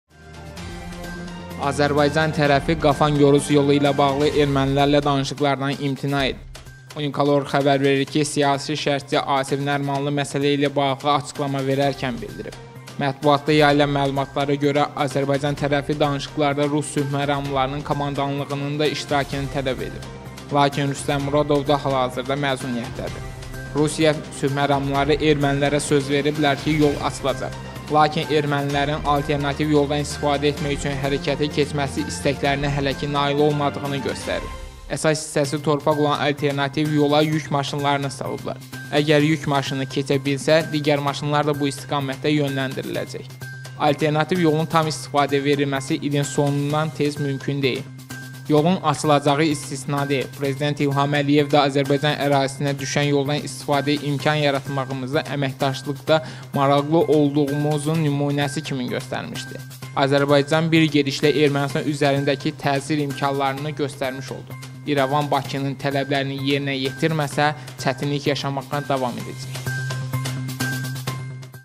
Azərbaycan ordusu yolu AÇMAQDAN İMTİNA ETDİ: Bakı bu tələbi irəli sürdü / Ermənilər yalvarır
“Azərbaycan tərəfi Qafan-Gorus yolu ilə bağlı ermənilərlə danışıqlardan imtina edib”.
Unikal.Org xəbər verir ki, siyasi şərhçi Asif Nərimanlı məsələ ilə bağlı açıqlama verərkən bildirib. Mətbuatda yayaılan məlumatlara görə, Azərbaycan tərəfi danışıqlarda rus sülhməramlılarının komandanlığının da iştirakını tələb edib. Lakin Rüstəm Muradov da hazırda məzuniyyətdədir.

"Rusiya sülhməramlıları ermənilərə söz veriblər ki, yol açılacaq. Lakin ermənilərin alternativ yoldan istifadə etmək üçün hərəkətə keçməsi istəklərinə hələki nail olmadığını göstərir: əsas hissəsi torpaq olan alternativ yola yük maşınını salıblar, əgər yük maşını keçə bilsə, digər maşınlar da bu istiqamətə yönləndiriləcək; alternativ yolun tam istifadə verilməsi ilin sonundan tez mümkün deyil.

Yolun açılacağı istisna deyil: Prezident İlham Əliyev də Azərbaycan ərazisinə düşən yoldan isifadəyə imkan yaratmağımızı əməkdaşlıqda maraqlı olduğumun nümunəsi kimi göstərmişdi.

Azərbaycan bir gedişlə Ermənistan üzərindəki təsir imkanlarını göstərmiş oldu: İrəvan Bakının tələblərini yerinə yetirməsə, çətinlik yaşamağa davam edəcək".